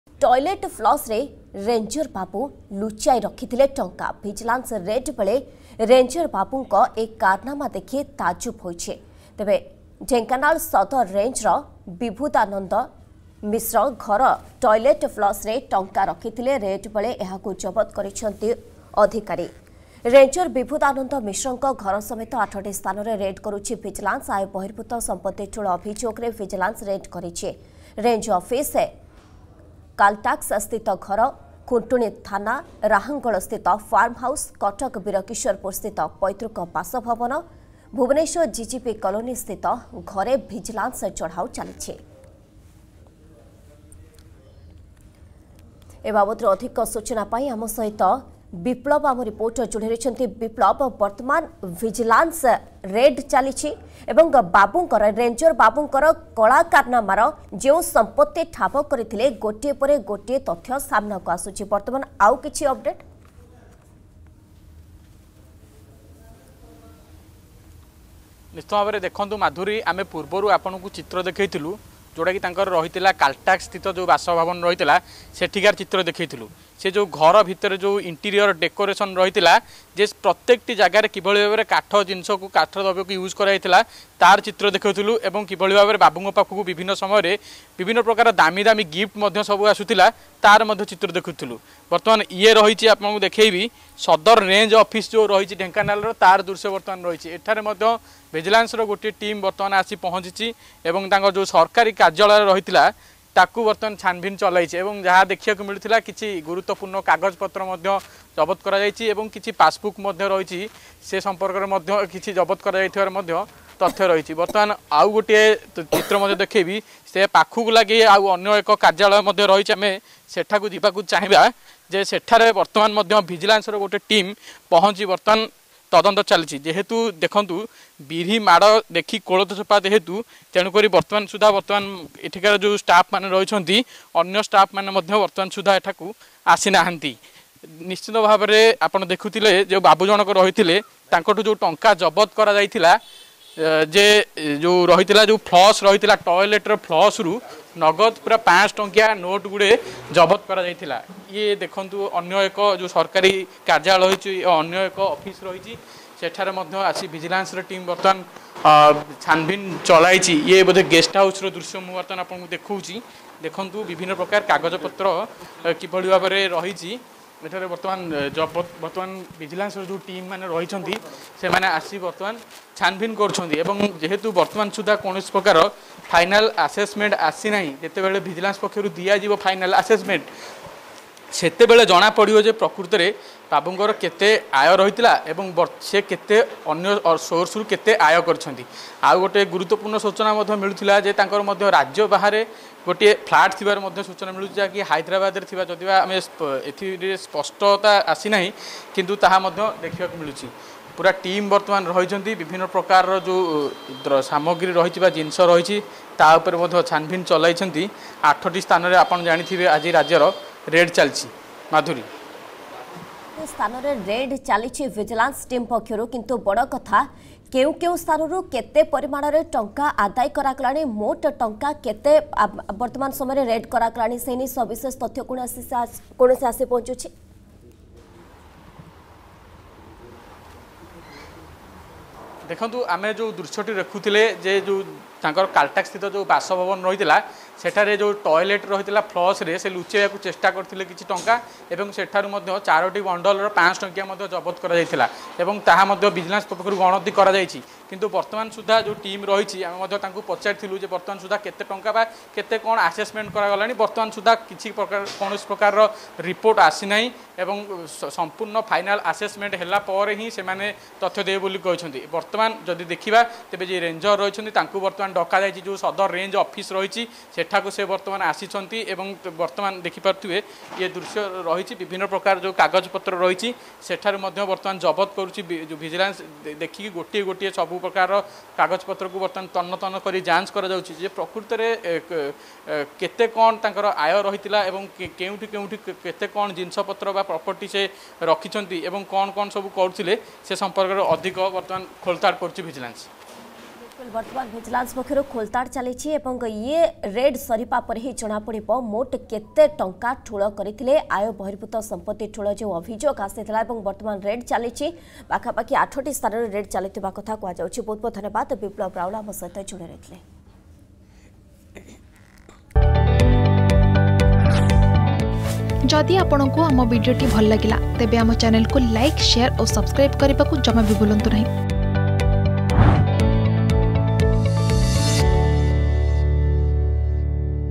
ଟଏଲେଟ୍ ଫ୍ଲସ୍‌ରେ ରେଞ୍ଜର ବାବୁ ଲୁଚାଇ ରଖିଥିଲେ ଟଙ୍କା || Dhenkanal Vigilance Raid
ଟଏଲେଟ୍ ଫ୍ଲସ୍‌ରେ ରେଞ୍ଜର ବାବୁ ଲୁଚାଇ ରଖିଥିଲେ ଟଙ୍କା । ଭିଜିଲାନ୍ସ ରେଡ୍ ବେଳେ ରେଞ୍ଜର ବାବୁଙ୍କ ଏ କାରନାମା ଦେଖି ତାଜୁବ ହୋଇଛି । ଢେଙ୍କାନାଳ ସଦର ରେଞ୍ଜର ବିଭୁଦାନନ୍ଦ ମିଶ୍ର ଘର ଟଏଲେଟ୍ ଫ୍ଲସ୍‌ରେ ଟଙ୍କା ରଖିଥିଲେ । ରେଡ୍ ବେଳେ ଏହାକୁ ଜବତ କରିଛନ୍ତି ଅଧିକାରୀ ।